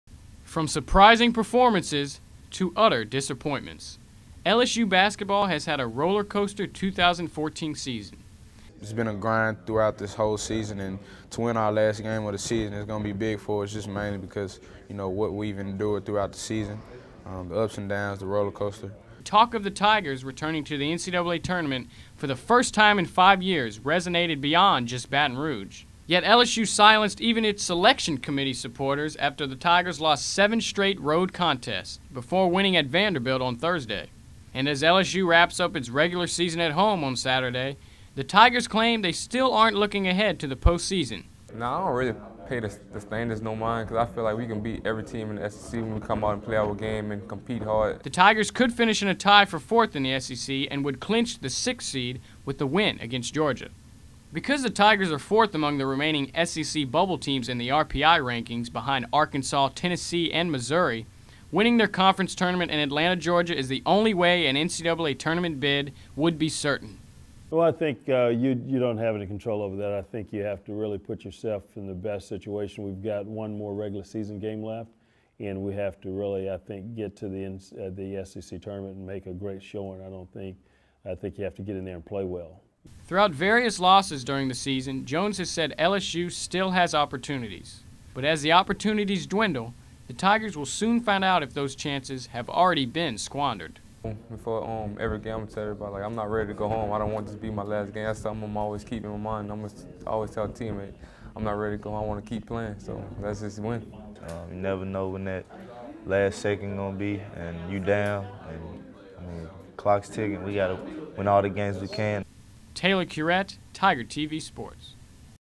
LSU Basketball Postseason Chances
As the Tigers host Georgia in their last regular season game, they look back on their opportunities to make an NCAA Tournament argument. Reporter Taylor Curet discusses LSU's chances for a Big Dance bid that depends on their SEC Tournament performance next week.